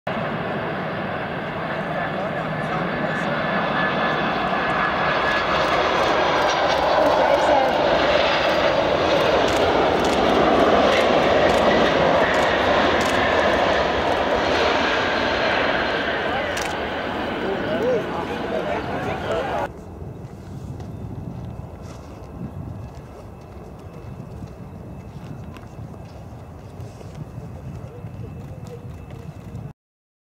Ed Force One [TF-AAK] landing at Munich Intl. airport
On Saturday, 28th of May 2016 the so called "Ed Force One", which is actually an Air Atlanta Icelandic Boeing 747 with registration TF-AAK brought the british band "Iron Maiden" for their concert to Munich. Seen here landing on runway 26L.